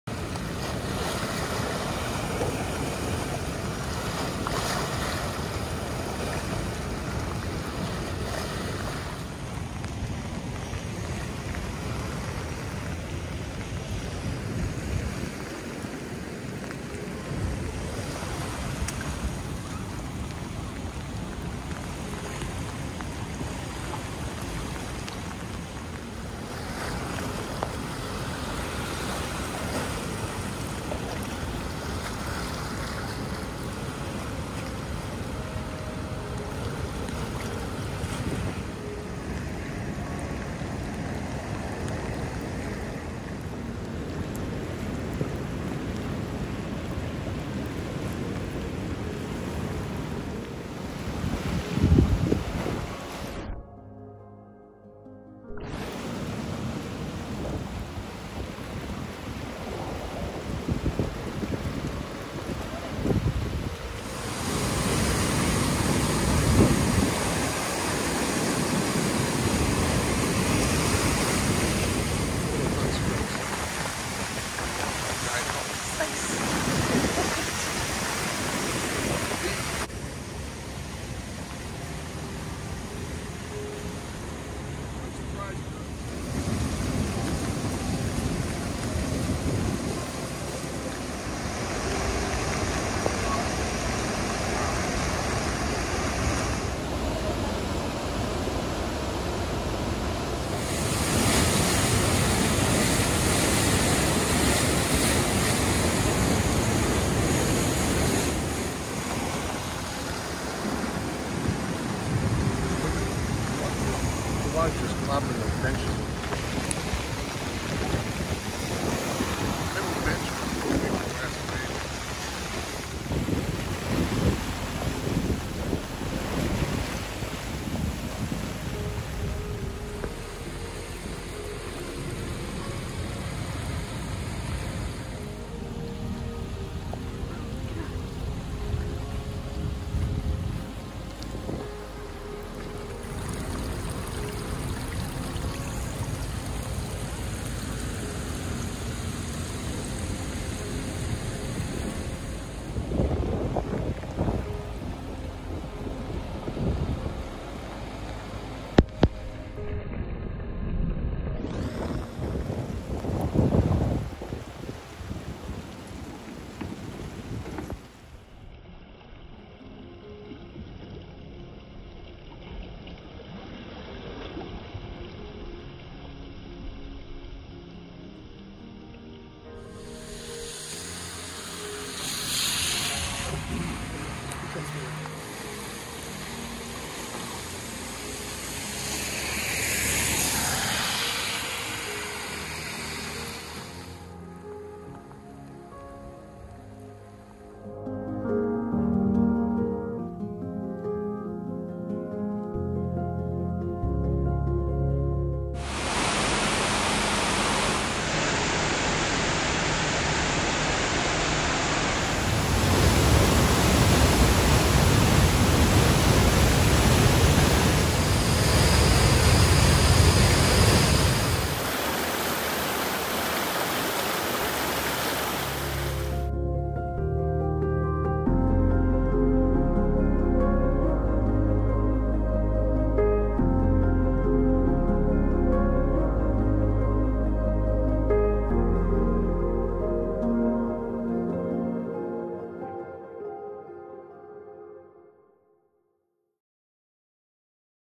December 14th flooding in Camden
High tide and rain event flooding throughout the Megunticook River Watershed.